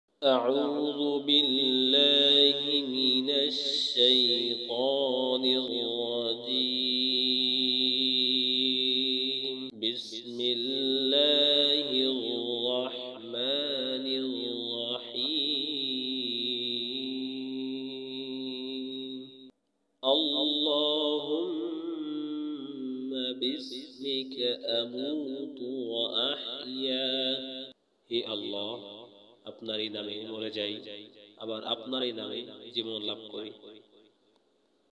(Du a before going to sleep) নিদ্রা যাওয়ার আগে দুআ
Dua before going to sleep
নিদ্রা যাওয়ার আগে দুআ

Pronunciation - Allahumma bismika amutu wa ahiya.
উচ্চারণ- আল্লাহুম্মা বিসমিকা আমুতু ওয়া আহইয়া।

Meaning: O Allah! I die in your name and live again in your name.
অর্থ : হে আল্লাহ! আপনারই নামে মরে যাই আবার আপনারই নামে জীবন লাভ করি।